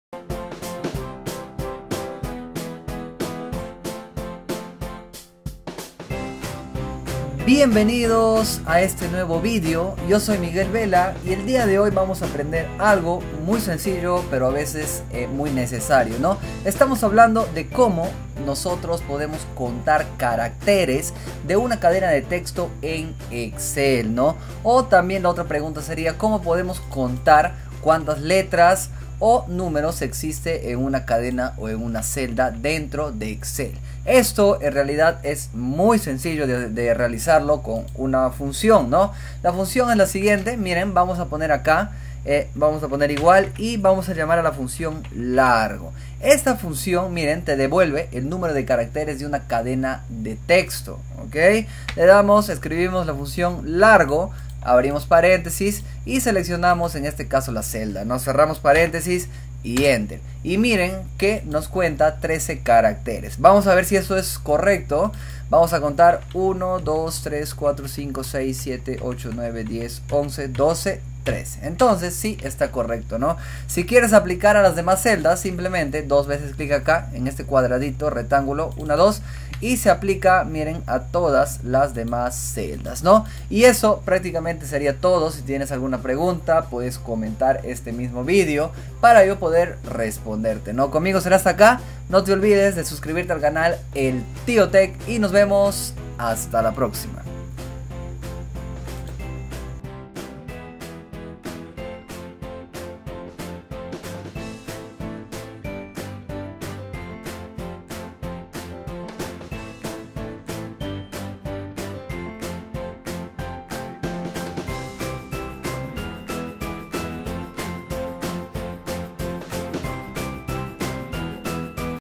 Como contar caracteres de una celda en Excel
Aprende a como contar caracteres de una celda en Excel. contar el número de letras y números que tiene una celda.